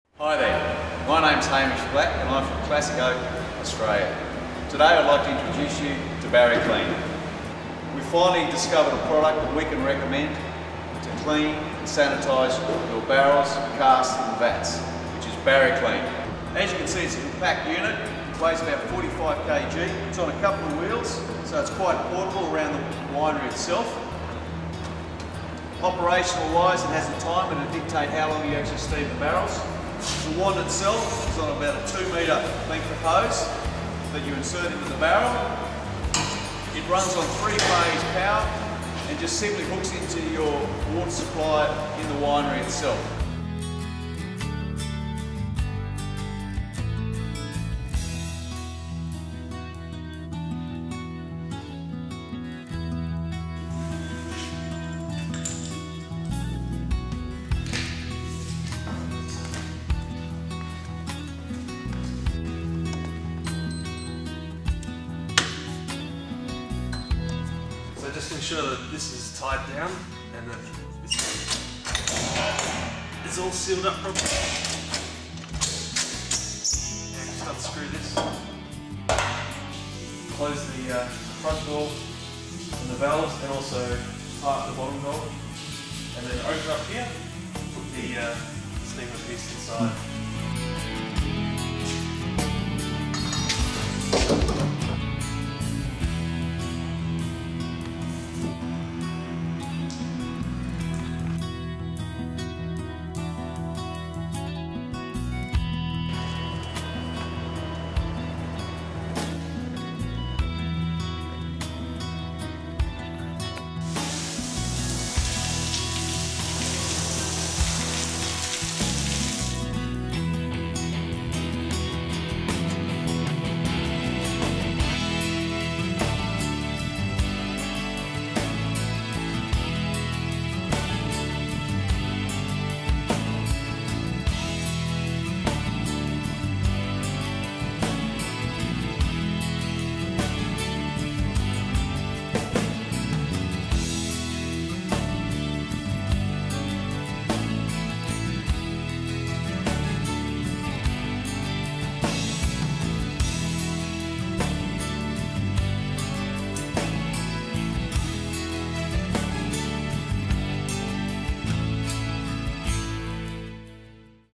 Barriclean®, générateur mobile de vapeur pour barriques par Bouyoud Distribution
Barriclean® est un générateur mobile de vapeur à température et débit constants pour désinfecter, nettoyer, détartrer vos barriques, foudres, cuves et accessoires. Disponible en 4 modèles, le choix de votre Barriclean® s'effectue en fonction du nombre de barriques quotidiennes ou de la quantité de grands contenants à laver.